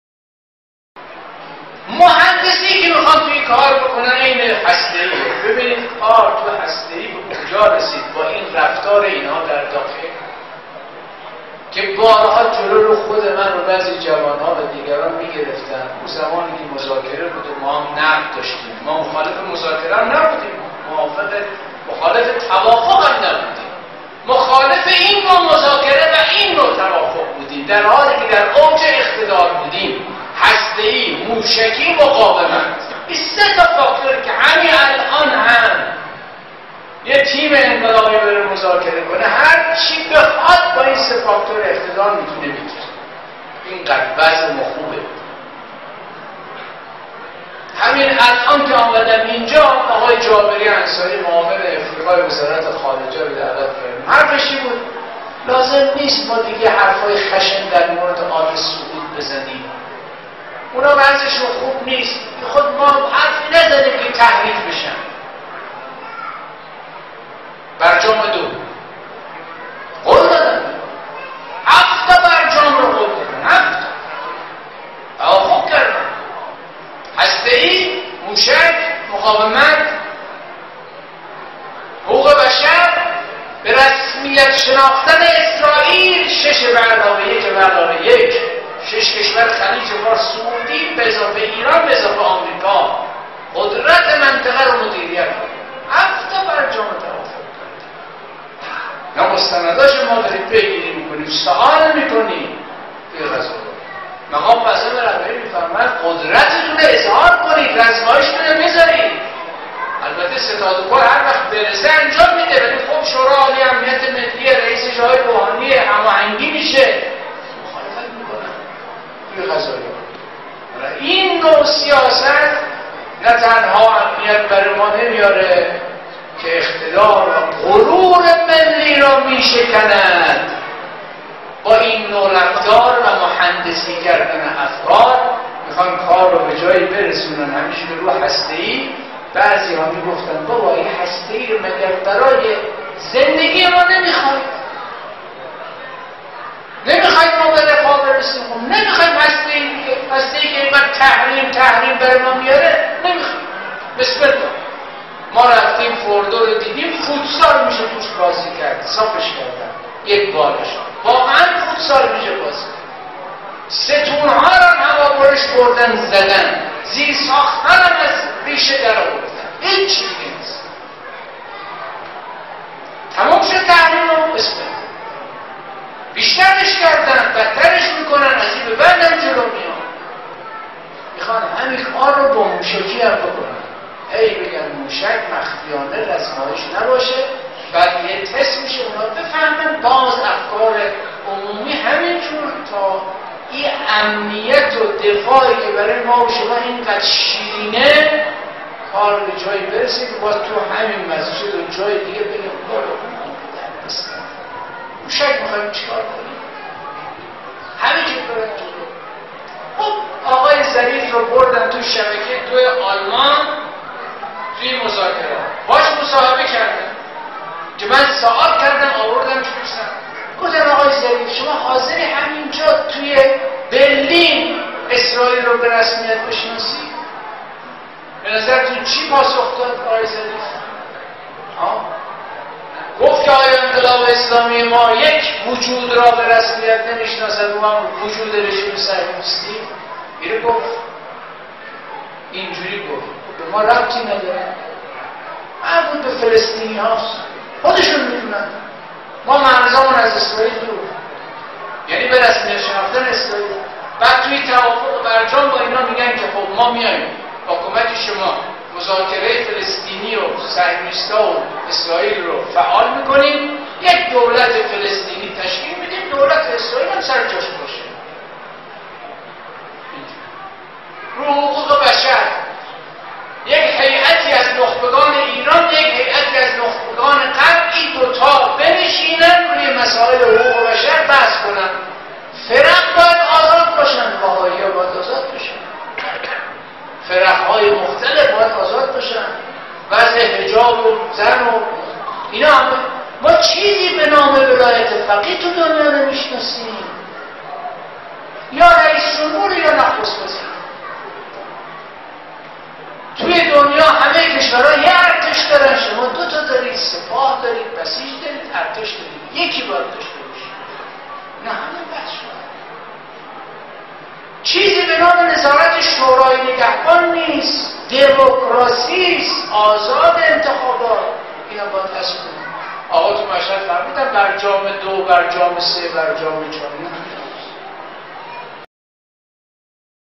توافق های پنهان برجام از زبان کریمی قدوسی
گزیده ای از سخنرانی جواد کریمی قدوسی نماینده محترم مردم مشهد مقدس در مجلس شورای اسلامی